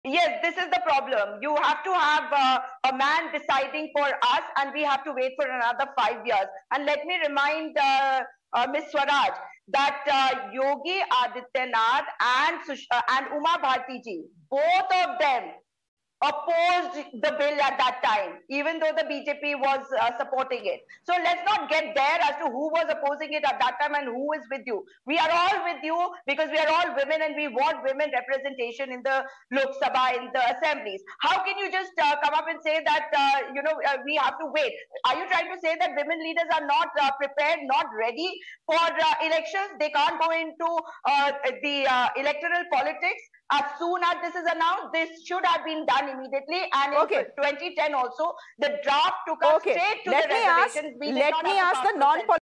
Why does the Women Reservation Bill not go straight to Reservation?
Why should it have to go through steps of census publication and delimitation before implementing Reservation?

Video courtesy: @mirrornow